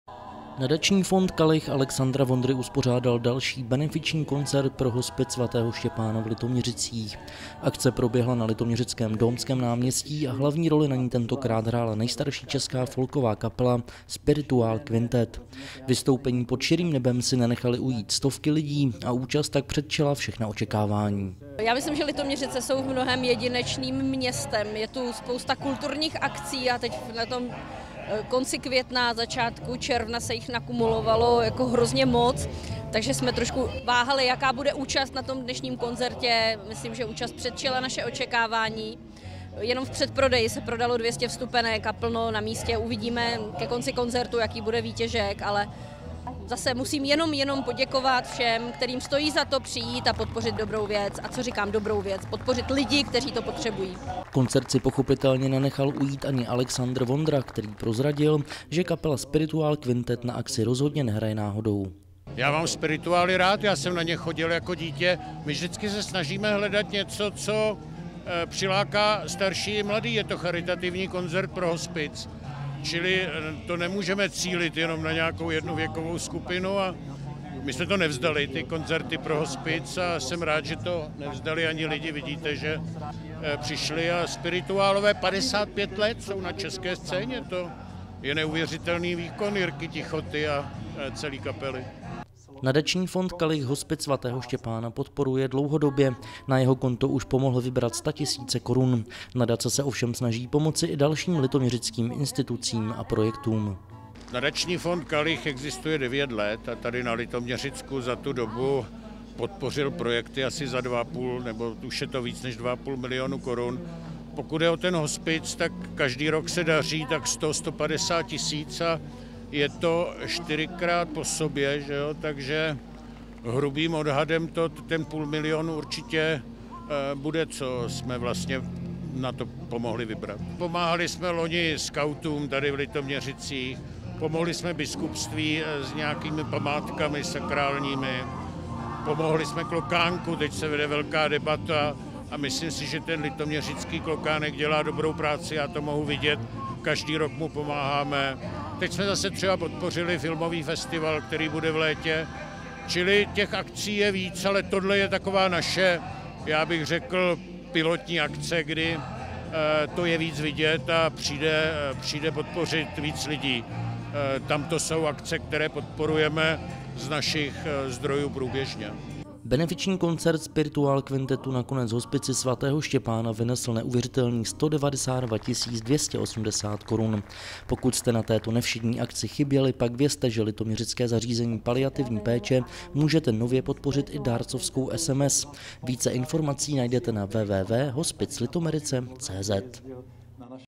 LITOMĚŘICE: Spirituál kvintet hrál pro hospic
Nadační fond Kalich Alexandra Vondry uspořádal další benefiční koncert pro Hospic sv. Štěpána. Akce proběhla na litoměřickém Dómském náměstí a hlavní roli na ní tentokrát hrála nejstarší česká folková kapela – Spirituál kvintet.